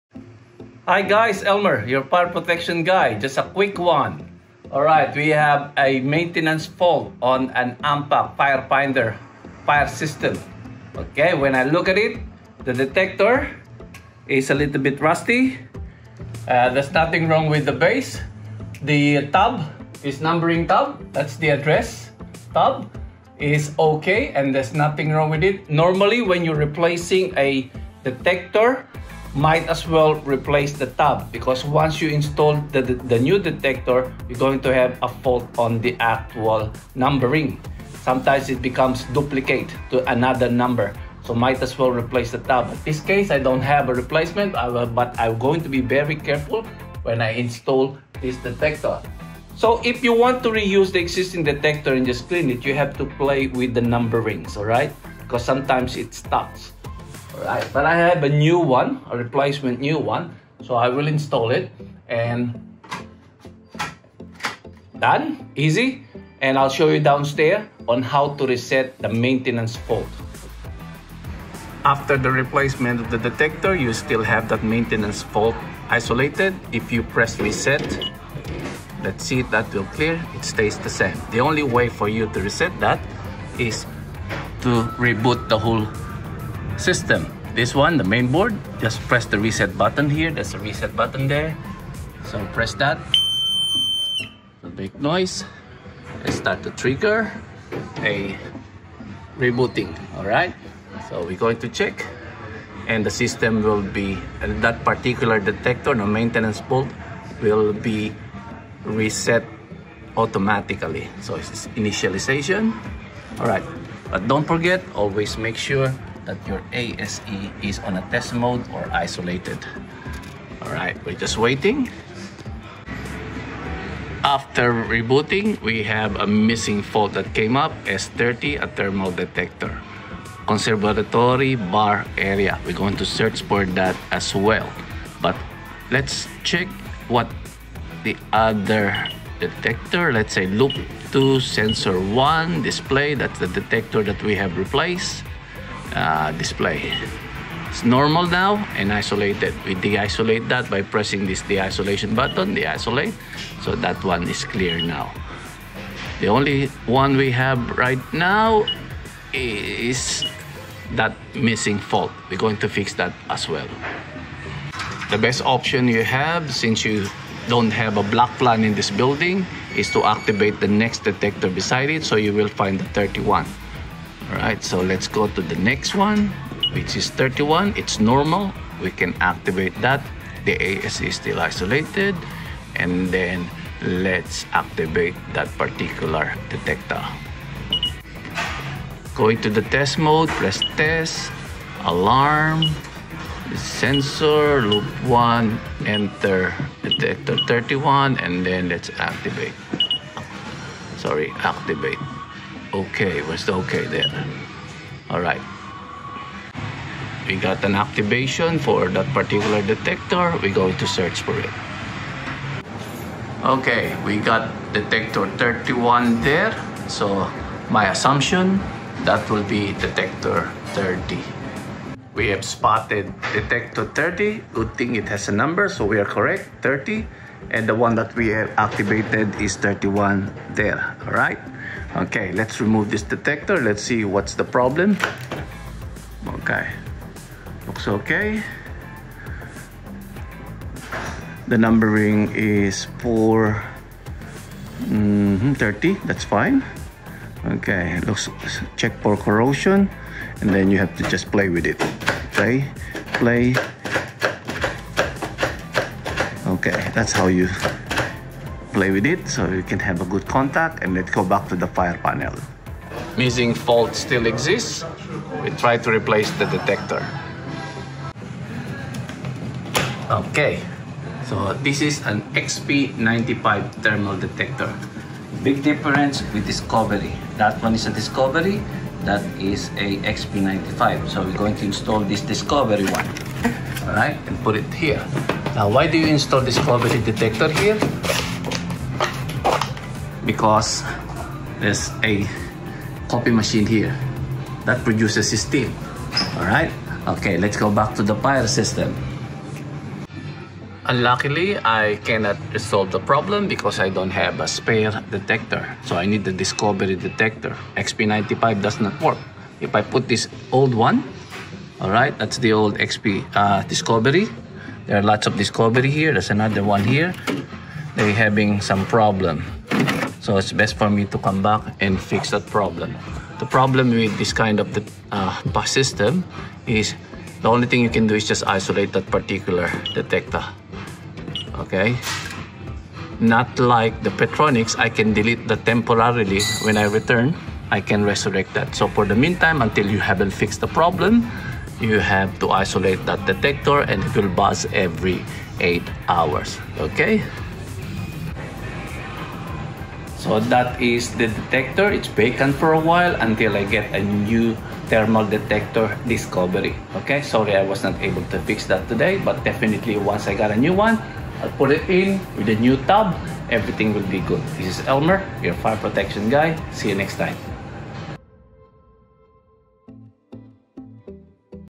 Basic Fault Finding Technique and Repair - Ampac Fire Finder & XP95
Attending a call due to maintenance fault. Easy way to look for the detector location and resetting this type of fault. But all not well as expected, another fault came out.